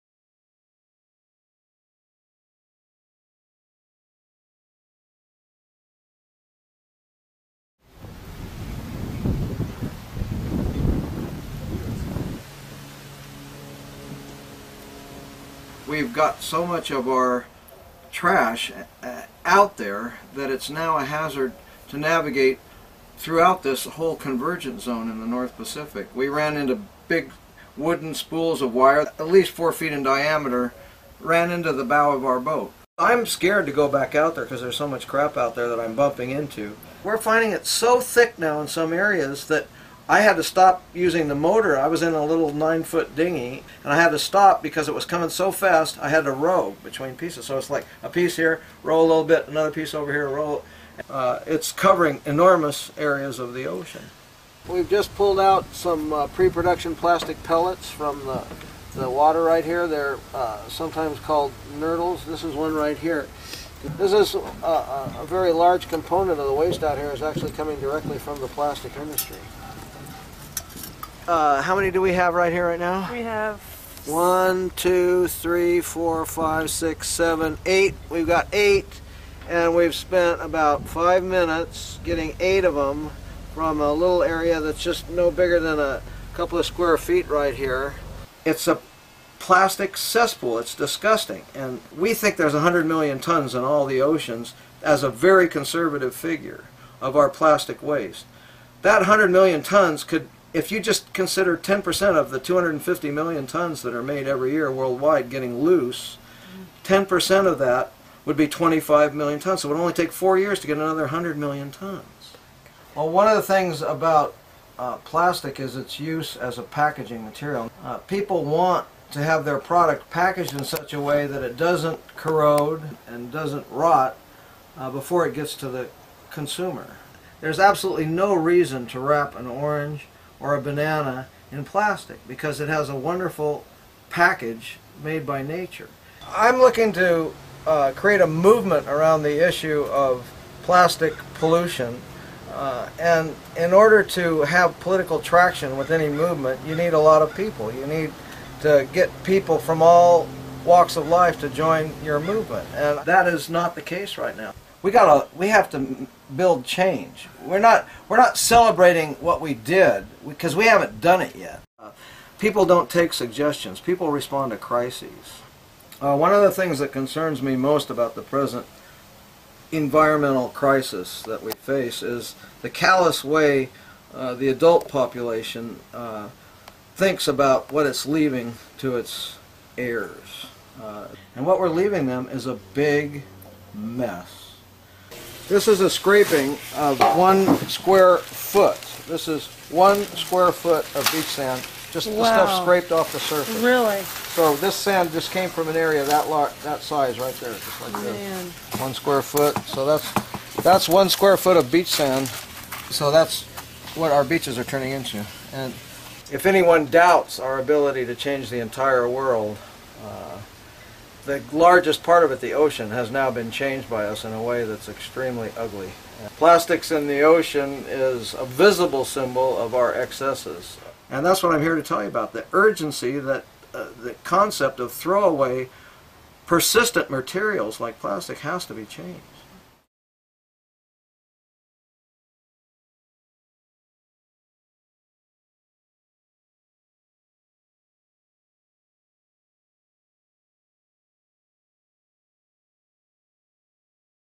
Charles Moore - Building Change
Captain Charles Moore, founder of the Algalita Institute, was the first person to widely recognize the existence of the "Great Garbage Patch" in the North Pacific Gyre.  Capt. Moore discusses the impact of plastics on the ocean, our callous environmental legacy, and the need for change.